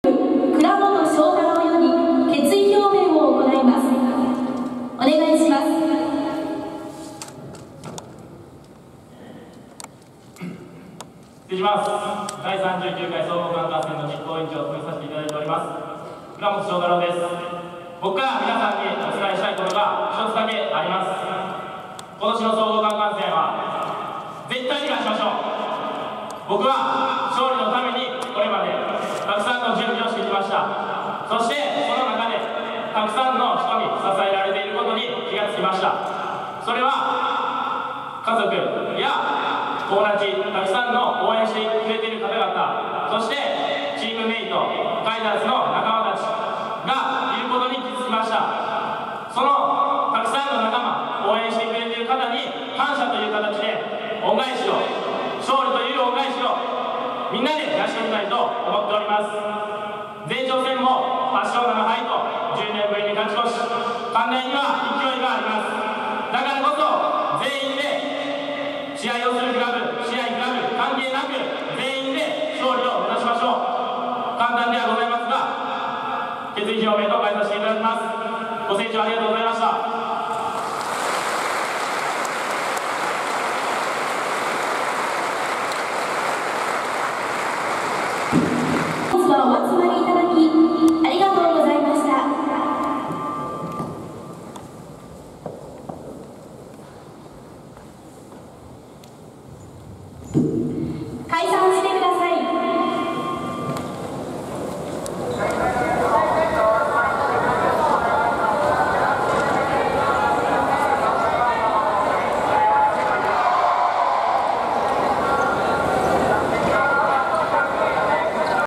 総合関関戦 前夜祭 2016【３完】決意表明
千里セルシー2016年6月5日雨天中止のため
千里山キャンパス 千里山中央体育館にて
2016年6月16日（木）総合関関戦前夜祭を開催。
今年は関大が前哨戦10年ぶりに勝ち越しを決めており
熱い応援が繰り広げられました！